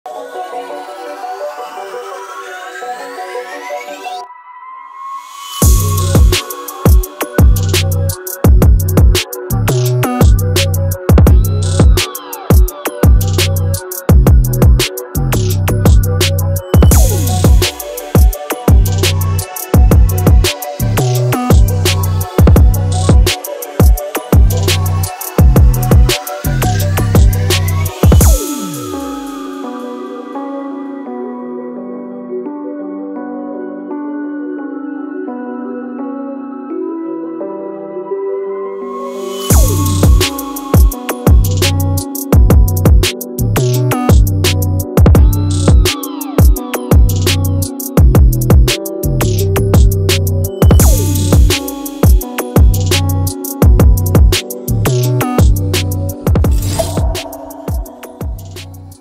PORSCHE CAYENNE I PAINTING WORK I KARSENZ TOTAL CAR CARE PVT LTD I CHENNAI I
Its about Painting & Monogram Work of Porsche Cayenne done at Karsenz Total Car Care Pvt Ltd, Chennai.